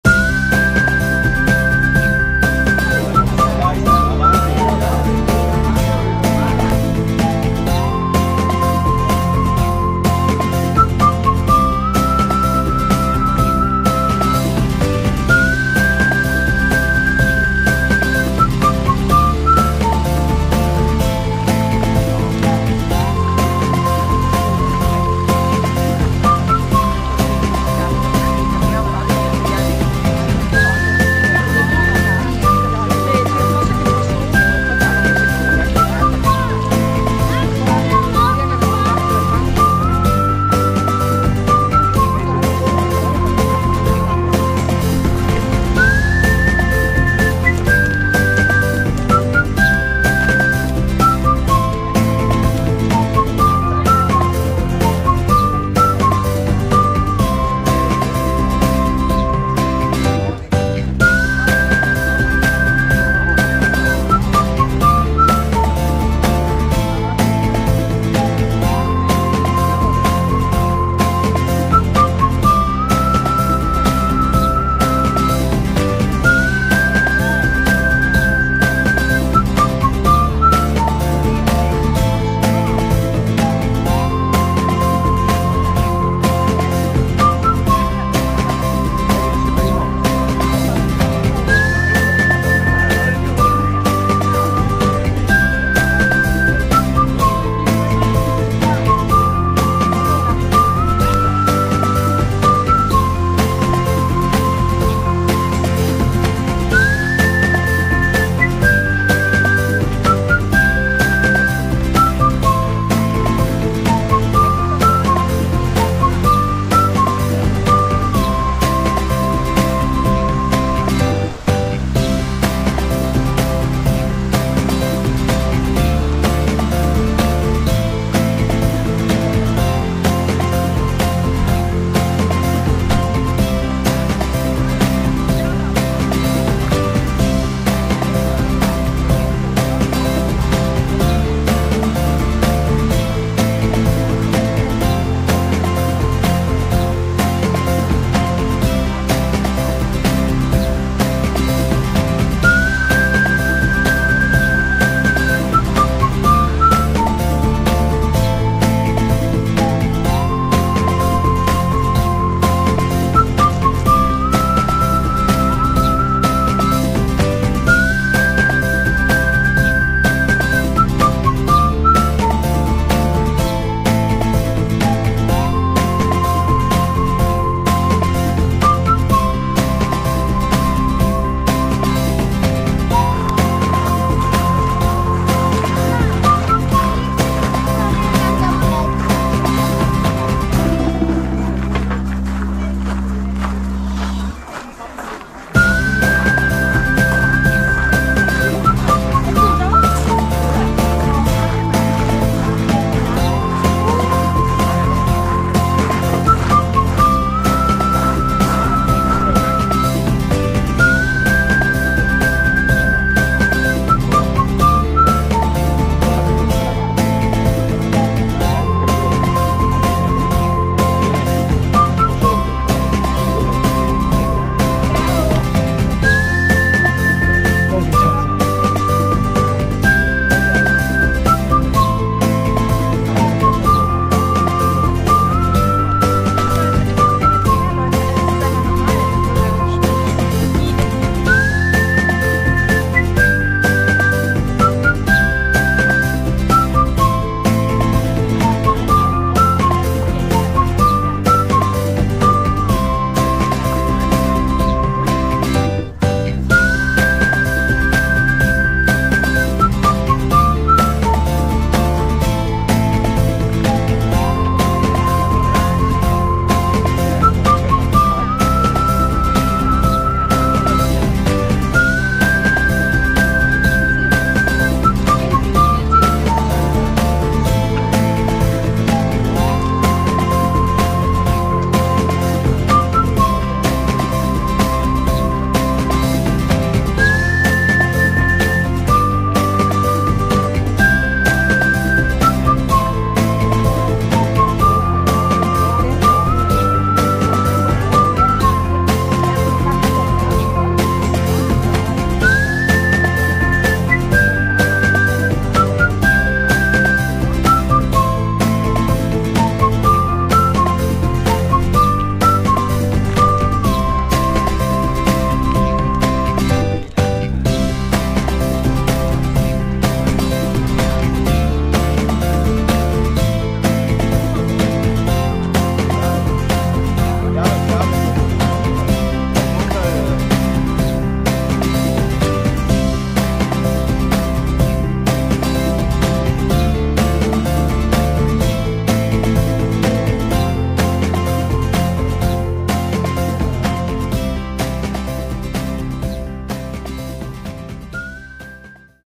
Camminata col sindaco 29 ago 2024 Ponteggio
Camminata col sindaco con partenza dal borgo di Ponteggio,buona partecipazione, con oltre duecento persone. Presente anche una tv tedesca ed il corrispondente della Tribuna di Treviso.